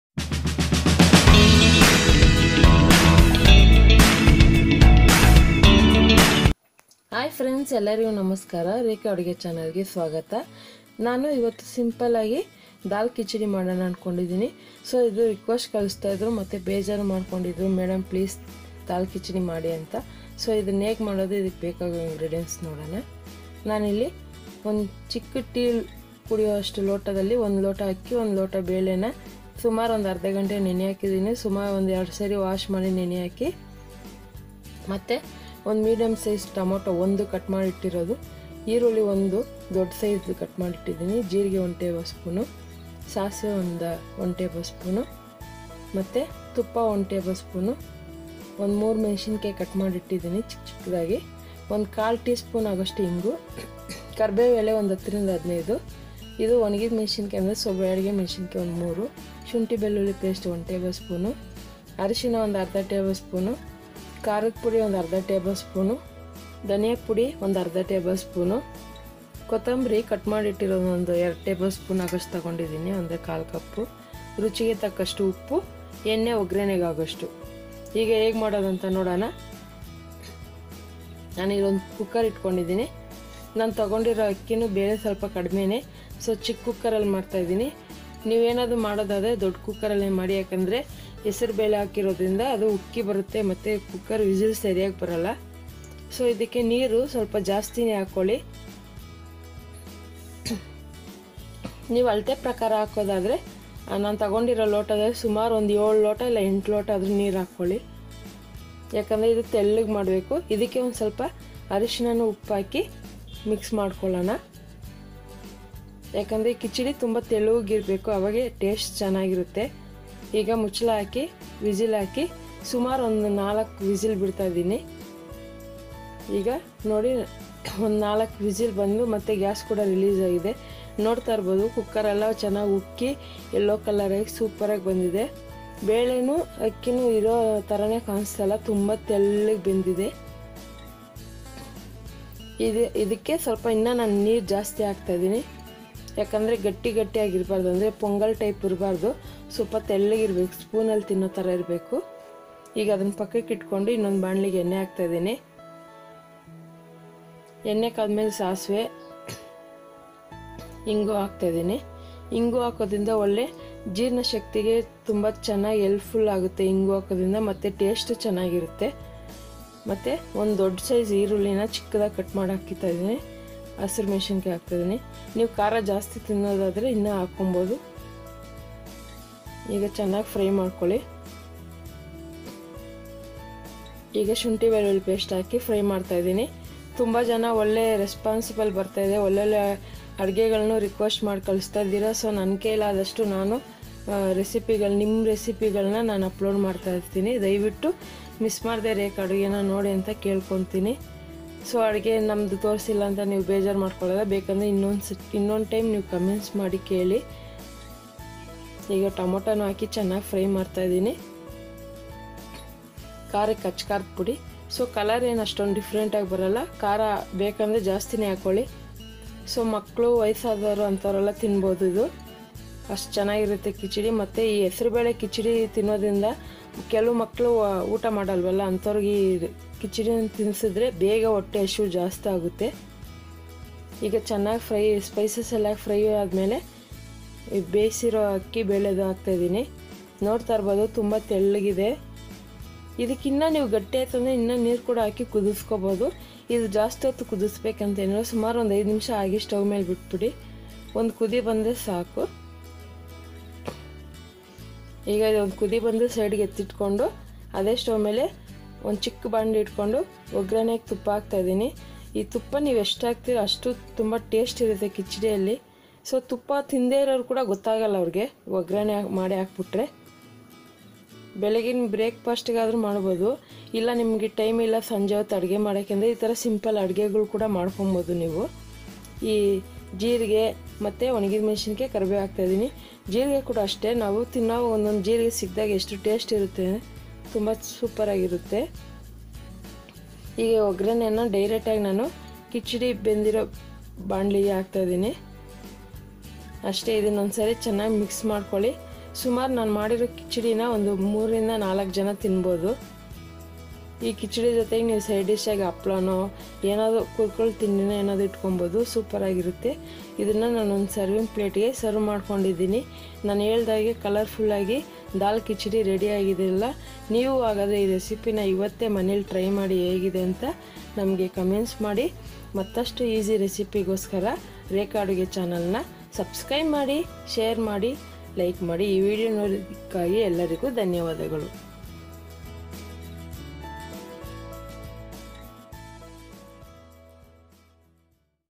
Dal Khichdi in Kannada | ಬೆಳೆ ಕಿಚಡಿ | Moong Dal Khichdi Recipe in Kannada | Rekha Aduge
Watch and learn how to spicy make Dal Khichdi recipe in Kannada / bele Kichdi recipe in Kannada / Moong Dal Khichdi recipe in Kannada / Healthy Dal Khichdi recipe in Kannada.